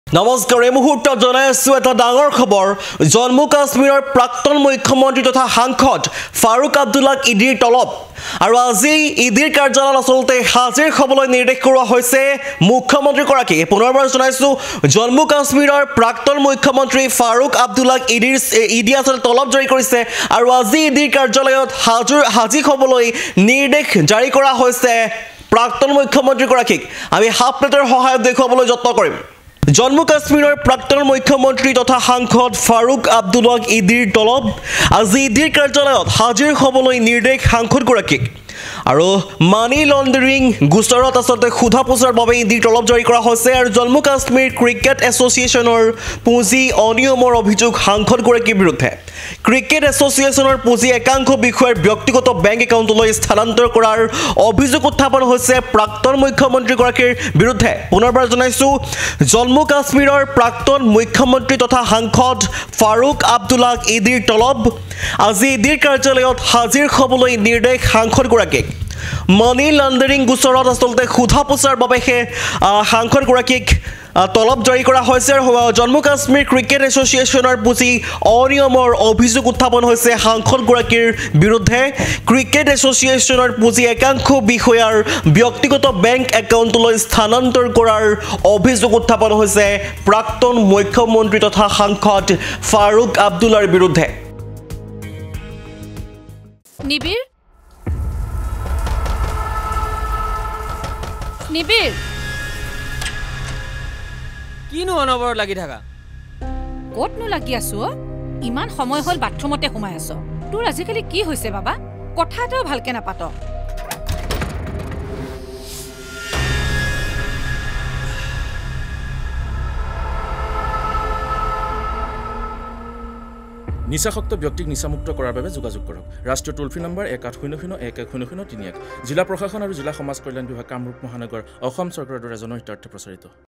জম্মু-কাশ্মীৰৰ প্ৰাক্তন মুখ্যমন্ত্ৰী তথা সাংসদ ফাৰুক আব্দুলাক EDৰ তলব
আজি EDৰ কাৰ্যালয়ত হাজিৰ হ'বলৈ নিৰ্দেশ সাংসদ গৰাকীক